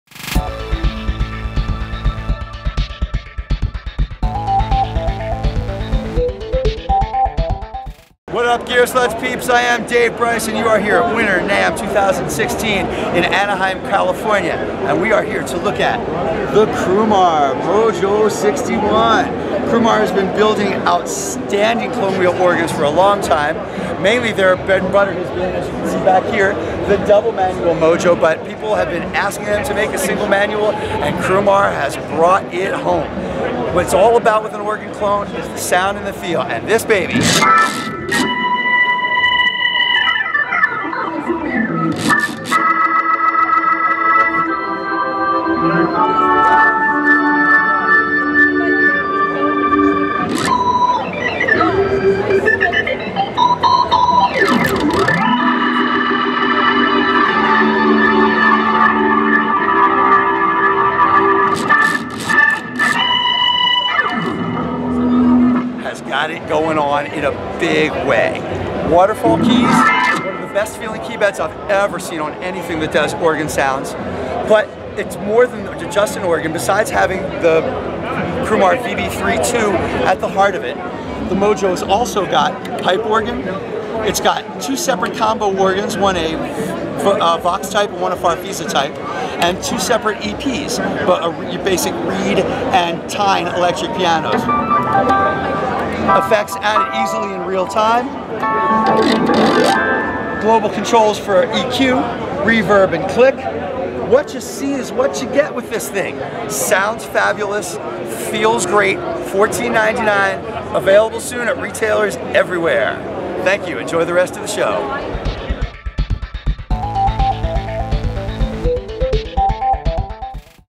Crumar Mojo 61 Organ - Gearslutz @ NAMM 2016
Hands-on with the new Crumar Mojo 61 at the 2016 Winter NAMM Show in Anaheim, California.